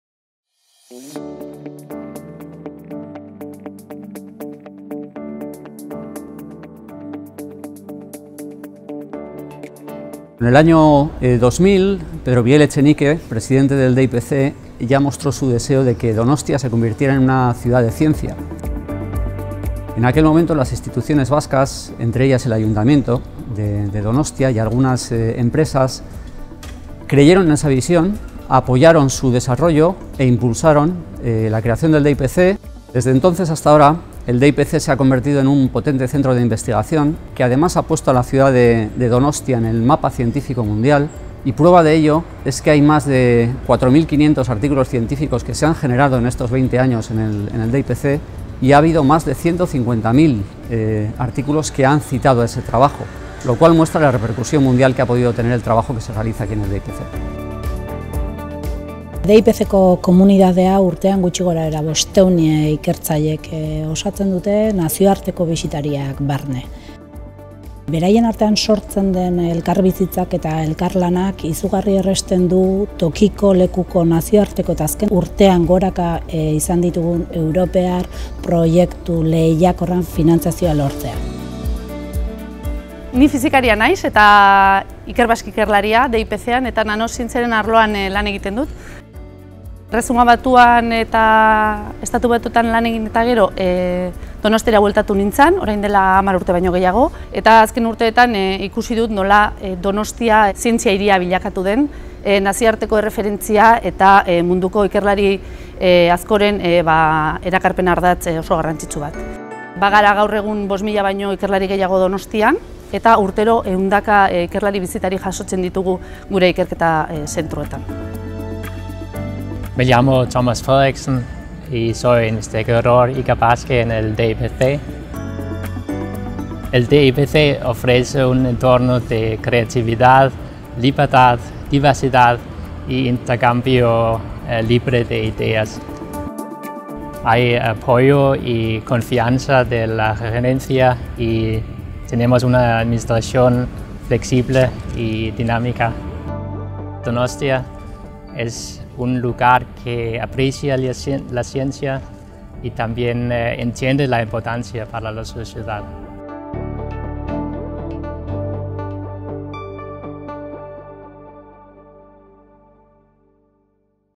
DIPC | Premio Reconocimiento DonostiaINN 2020
El DIPC (Donostia International Physics Center) se ha adjudicado el Premio 'Reconocimiento DonostiaINN 2020' por su recorrido, por su apuesta por la innovación, su enfoque global y su vinculación con la ciudad.

El DIPC (Donostia International Physics Center) fue inaugurado en abril del 2000 con el objetivo de promocionar la investigación científica en el campo de la Física básica y aplicada en ámbitos de interés para la sociedad vasca y para el desarrollo científico internacional.